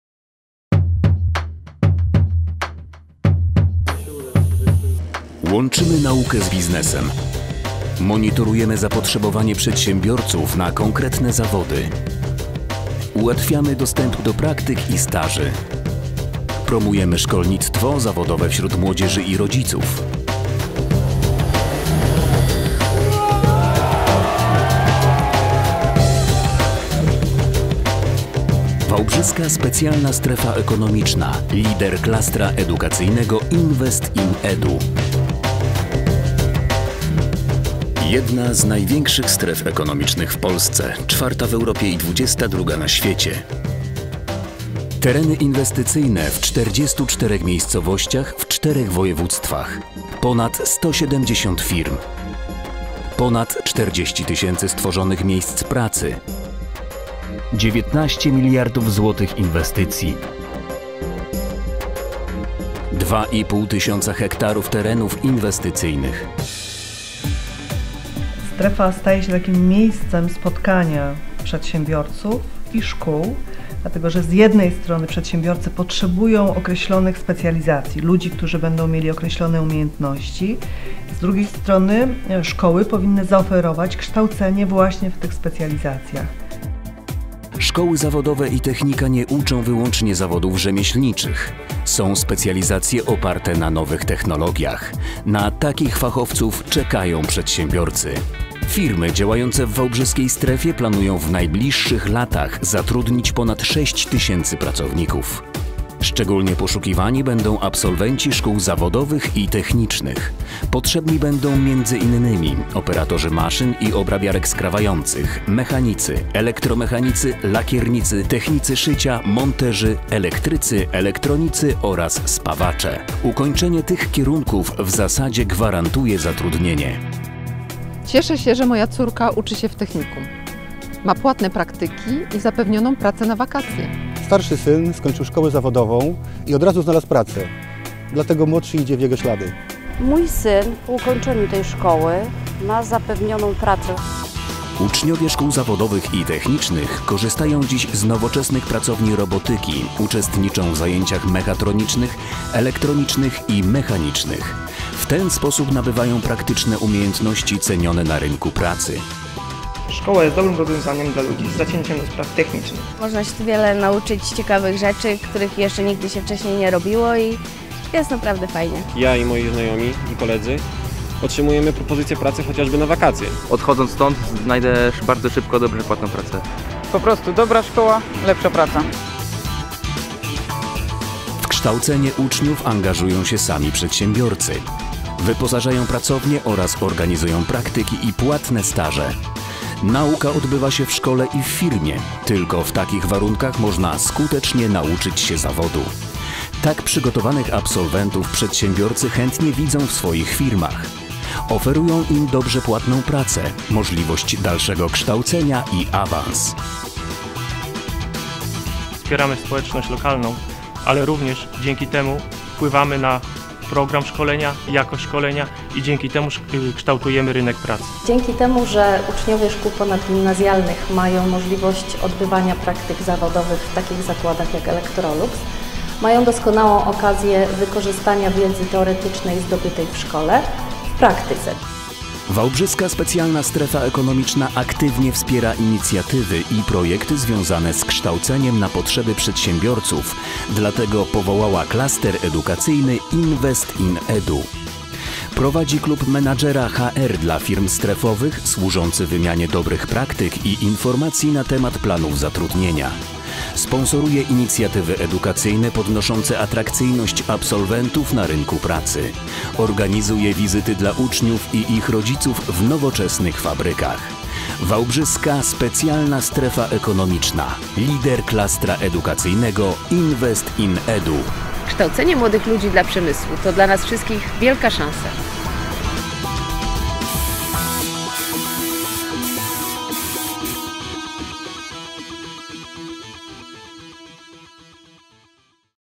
INVEST IN EDU long WSSE "Invest-Park"
Wałbrzyska Specjalna Strefa Ekonomiczna Invest-Park inwestuje w edukację zawodową